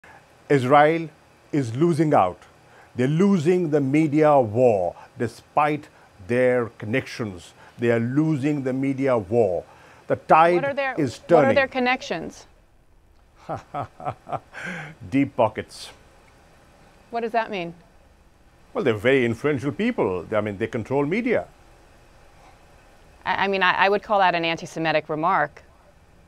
Pakistan foreign minister spouts antisemitism on CNN, gets called out by Bianna Golodryga
Pakistan’s foreign minister echoed a pair of antisemitic tropes on a CNN segment discussing the Israel-Gaza violence on May 21st.

In an interview with anchor Bianna Golodryga, Shah Mahmood Qureshi said that Israel is losing the “the media war” in its battle against Hamas, “despite their connections.”

Golodryga shot back, saying “What are their connections?”

Qureshi laughed, then said “Deep pockets.”

Golodryga pushed again, asking what he meant, and Qureshi answered: “Well they’re very influential people, they control media.”

“I would call that an antisemitic remark,” Golodryga responded.